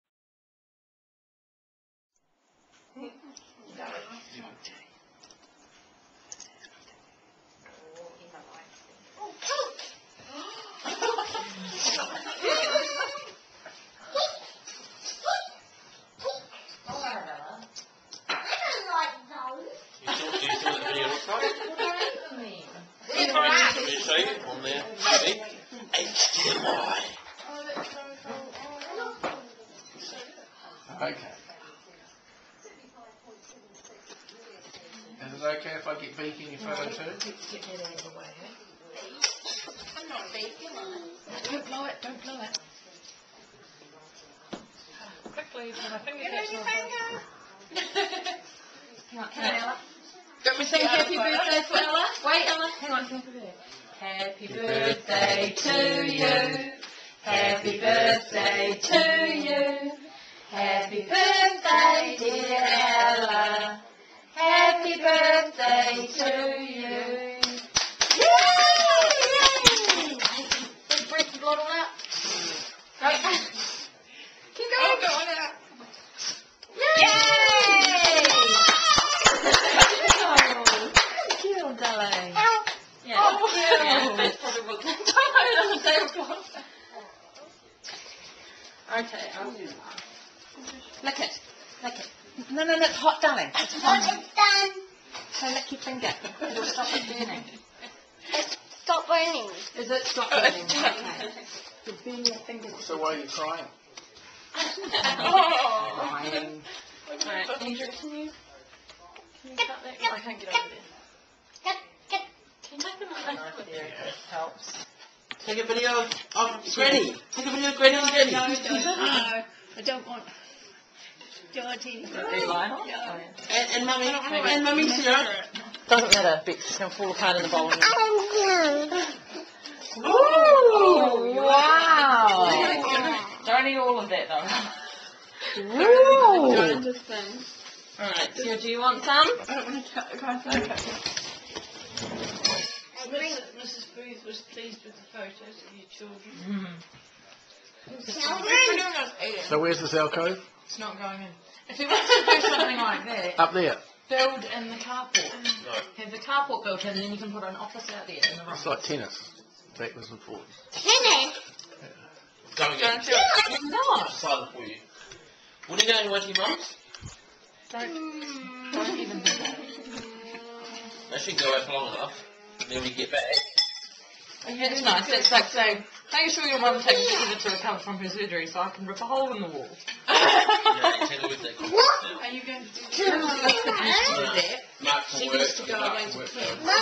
A princess turns five.
A dinner with family to celebrate Ella turning Five